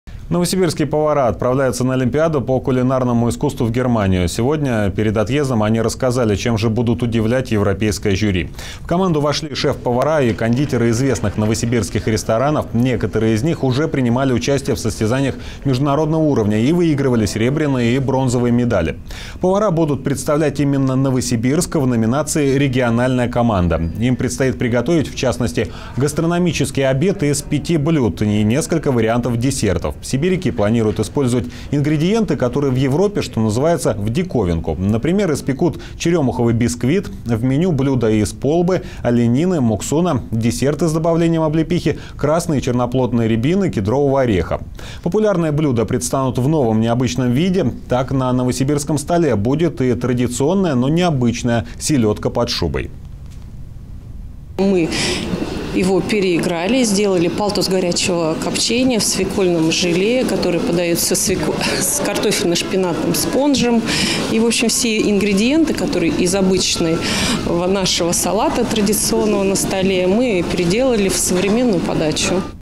Новосибирские повара едут на кулинарную Олимпиаду
Облепиховое мороженое, палтус под шубой. Новосибирские шеф-повара приготовили для кулинарной Олимпиады блюда с сибирским колоритом.